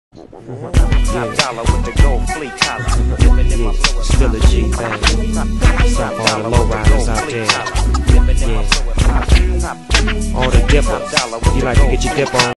Still Dippen
Dinky the dog cruising through Los Angeles, over the famous LA River.  With the beautiful  LA skyline backdrop.